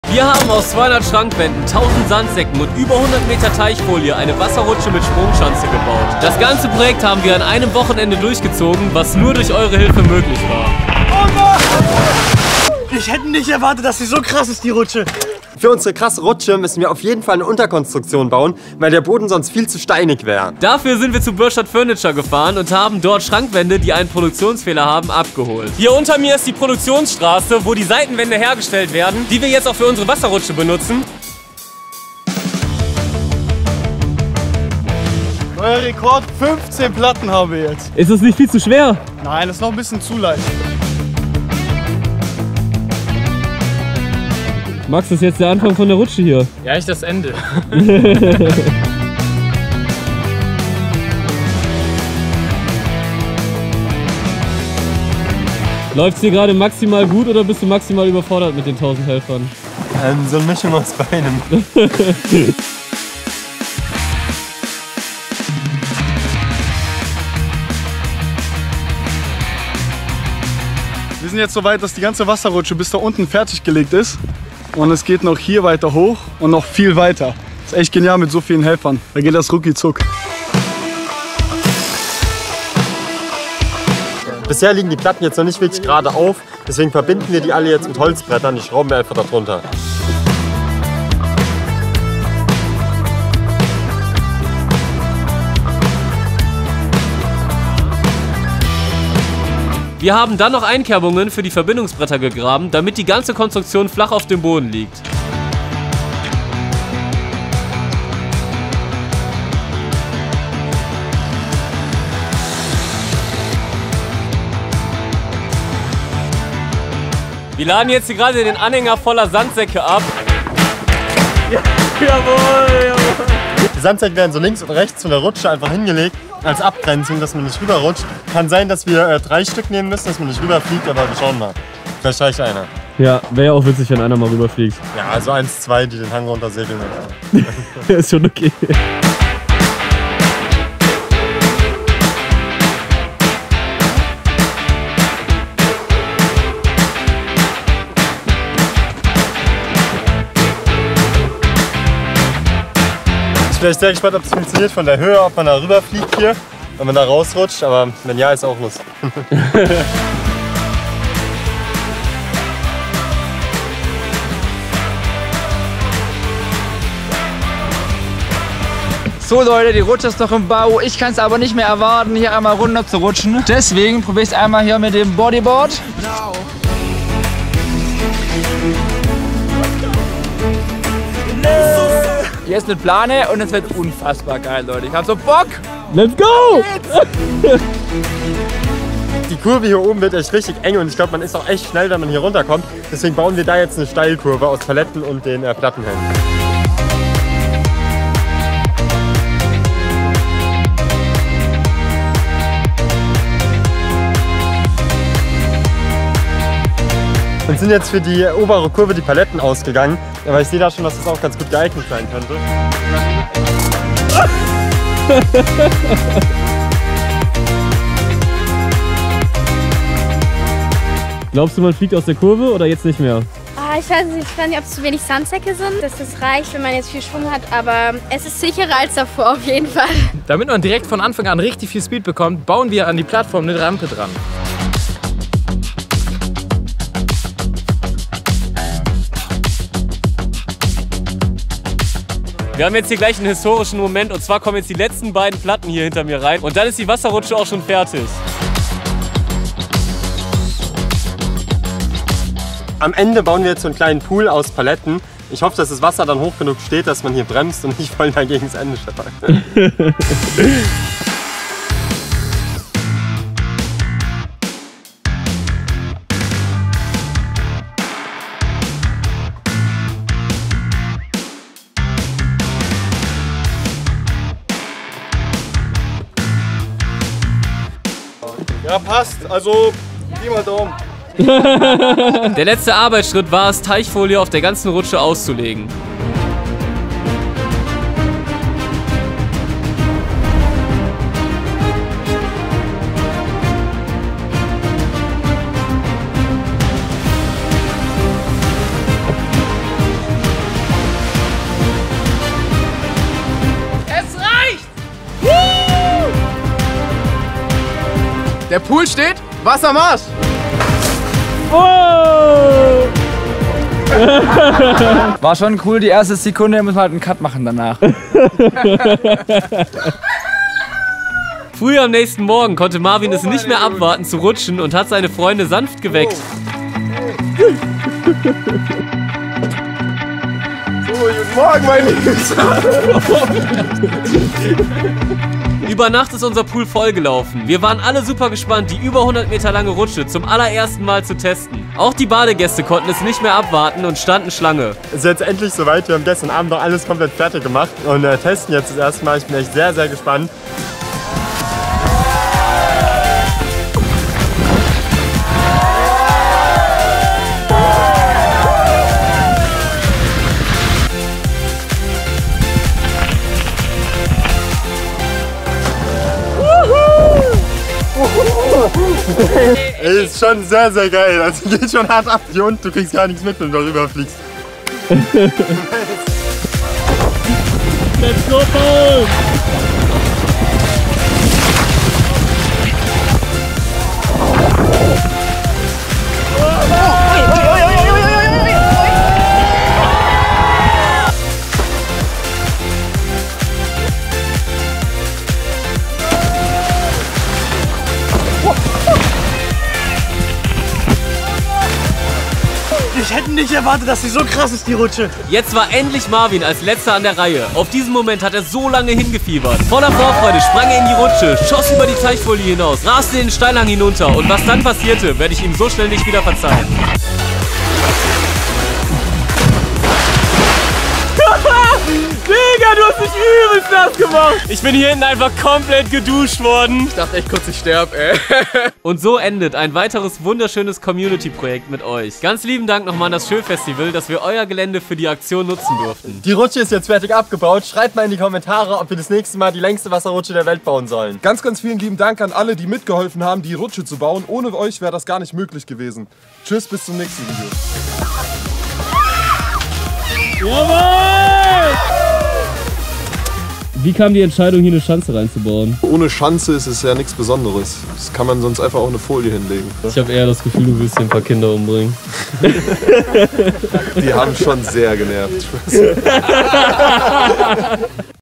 Wir bauen eine gigantische WASSERRUTSCHE mit 500 ZUSCHAUERN
Wir haben mit euch zusammen eine 120m lange Wasserrutsche gebaut. Das ganze Projekt war nur durch eure zahlreiche Unterstützung möglich!
Vielen Dank an alle, die mit dabei waren und an das Schö Festival, dass wir bei euch sein durften :)

Bis bald
Johannes, Daniel, Max und Julius

TikTok: The Real Life Guys

MIT "*" GEKENNZEICHNET = WERBUNG